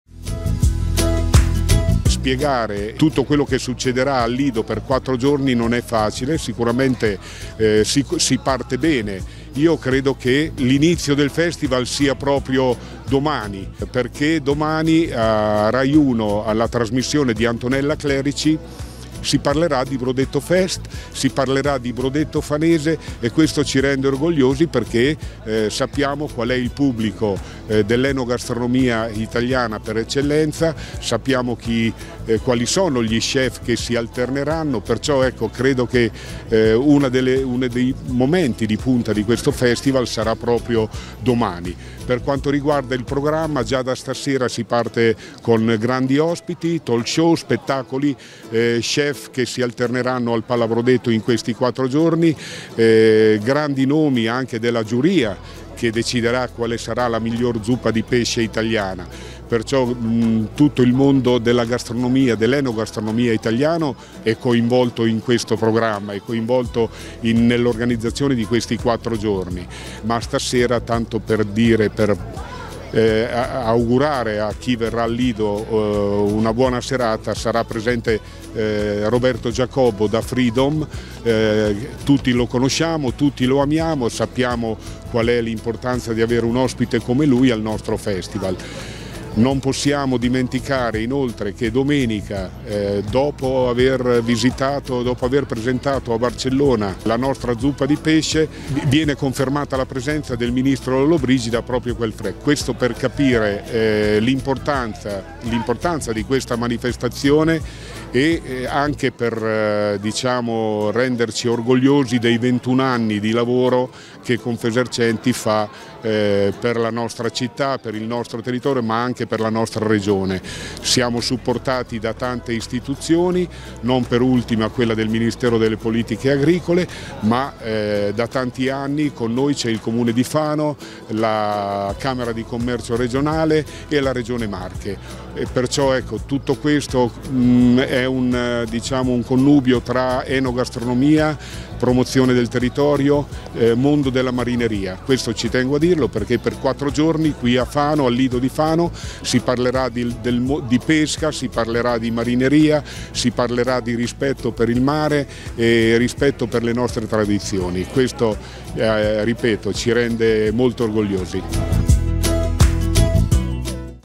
Inaugurato il BrodettoFest al Lido di Fano
Al via la 21ª edizione di BrodettoFest: l'evento inaugurato al palco centrale del Lido di Fano. Si parte con lo chef stellato Giuseppe Mancino e il conduttore di Freedom Roberto Giacobbo.